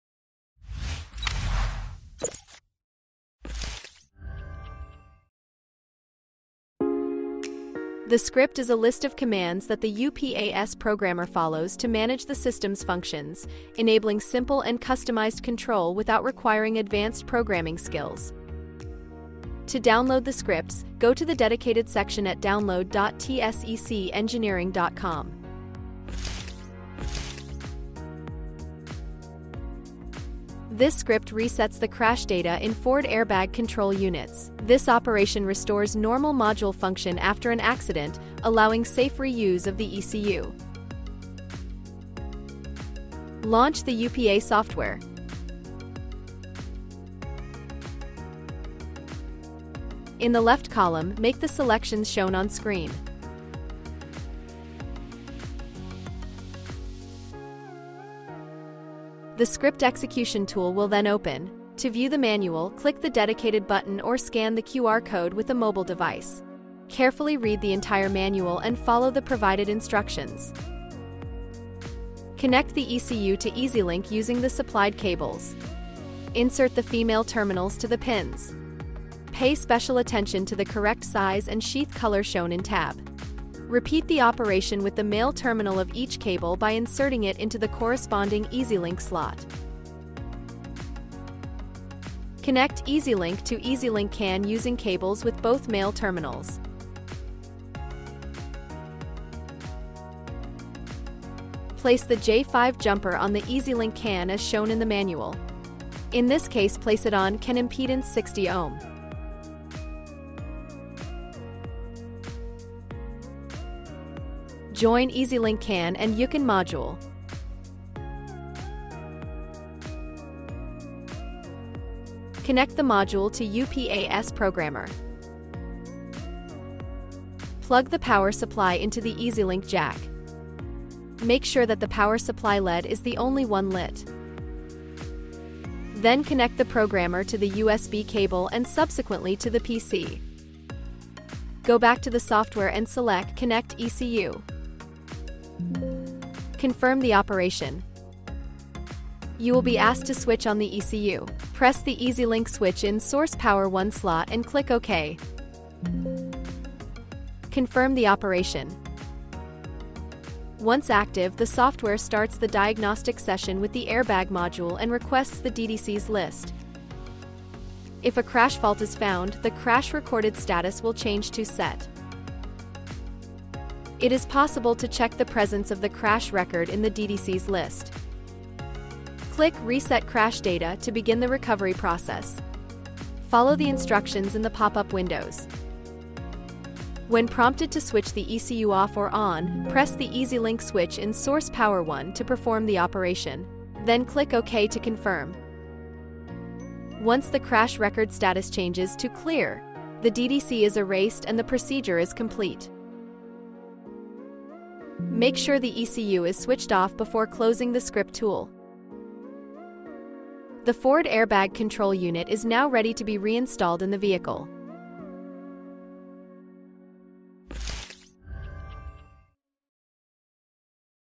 How to Reset Ford FoMoCo (Veoneer) Airbag Crash Data with UPA - S Programmer SCRIPT - 0011 & iSyLink
Scopri come eseguire lo SCRIPT-0011 per effettuare il reset dei dati crash sulle centraline Airbag Ford dotate di MCU Infineon SAK-TC222S-16F (Veoneer FoMoCo).

Con un’unica procedura automatizzata potrai ripristinare in sicurezza la funzionalità completa della tua ECU.

Caratteristiche principali:
Automazione totale: Reset dei dati crash con un solo click
Guida passo-passo Manuale integrato per operazioni semplici e sicure
Reset affidabile: Rimuove i dati di crash e riporta la centralina a uno stato pienamente operativo

Quando usarlo:
Dopo incidenti con attivazione dell’airbag
In presenza di errori persistenti sulla centralina

Applicazioni compatibili:
• EcoSport MK2: GN15-14B321-LB/LC/LD, LN15-14B321-BB/BC
• Fiesta MK7: H1BT-14B321-AF/AG/AH/AJ
• Ka+ MK3: J7BT-14B321-AC/AD
• Puma: L1TT-14B321-AB, N1BT-14B321-HA
• Ranger T6: JB3T-14B321-BE/FA
• Transit/Tourneo MK3: KK3T-14B321-AC
• Transit/Tourneo MK4: JK2T-14B321-AH/AJ, LT1T-14B321-DA, MK2T-14B321-CA/DA

Script disponibili con licenza Mensile • Trimestrale • Semestrale • Annuale • Perpetua

🔗 Per acquisti e personalizzazioni: accedi alla sezione UPAS Programmer → SCRIPTs dell’area riservata oppure su